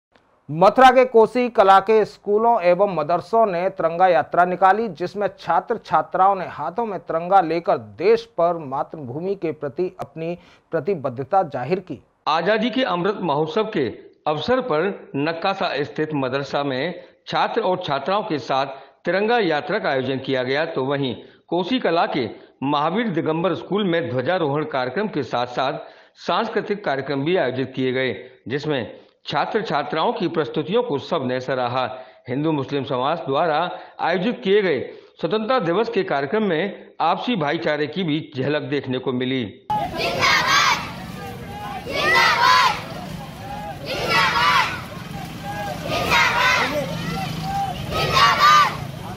कोसीकला में स्कूल और मदरसों के छात्र छात्राओं ने पूरे जोश के साथ मनाया स्वतंत्रता दिवस....
GnineNews- कोसीकला में स्कूल और मदरसों के छात्र छात्राओं ने पूरे जोश के साथ मनाया स्वतंत्रता दिवस.